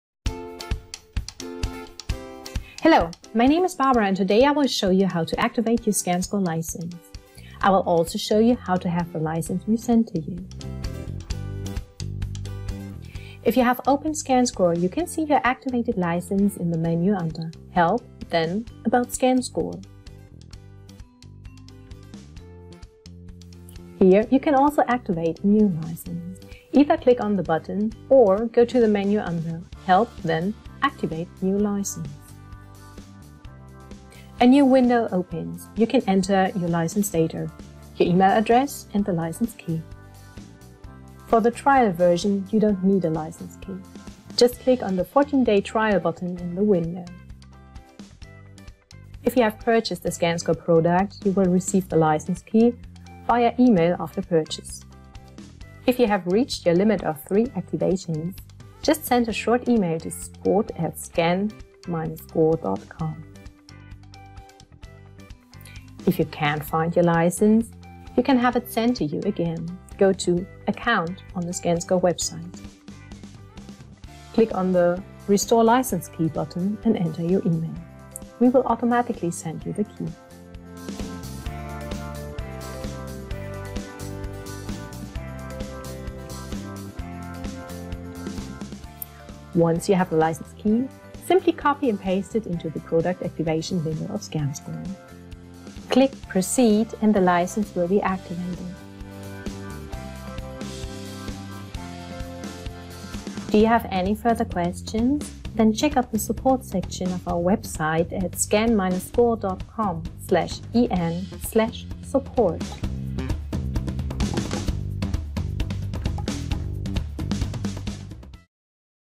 How to activate your ScanScore license
In this video tutorial we will show you how to activate your ScanScore license.

Content:
- Activate your license
- Show your activated ScanScore version
- Have your license resent to you